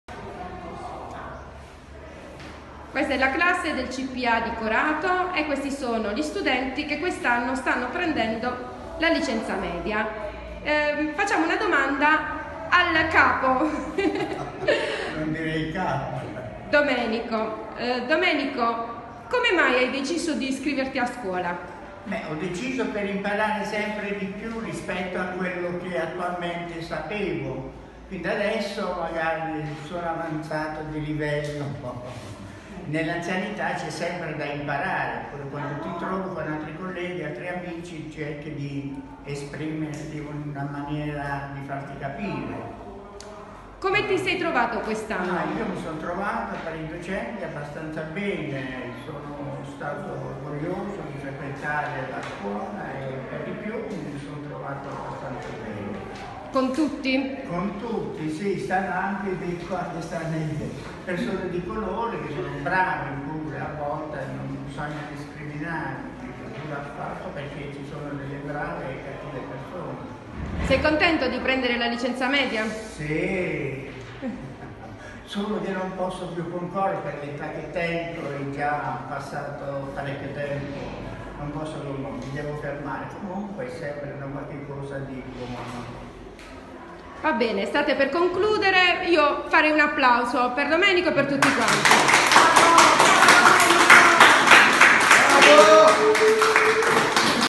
Licenza media a 83 anni per nonno Domenico: "Peccato non poter fare concorsi"
"Sono molto contento": Domenico Di Bartolomeo ha preso la licenza media a 83 anni. Il pensionato di Corato, che aveva abbandonato la scuola dopo la licenza elementare, ha affrontato l'esame con i suoi compagni del Cpia 1 di Corato. una scelta fatta "per i nipotini" e per poter leggere loro le storie della buonanotte. Il colpo d'occhio della foto di fine anno riscalda il cuore e riaccende le speranze. Nella scuola statale che garantisce a cittadini italiani e stranieri servizi e attività per l'educazione in età adulta, Domenico ha studiato italiano, matematica, scienze, francese e tecnologia, tutti i pomeriggi per un anno, dal lunedì al venerdì. "Peccato non poter concorrere: sono contento ma mi fermo qui". .  .A cura di Silvia Dipinto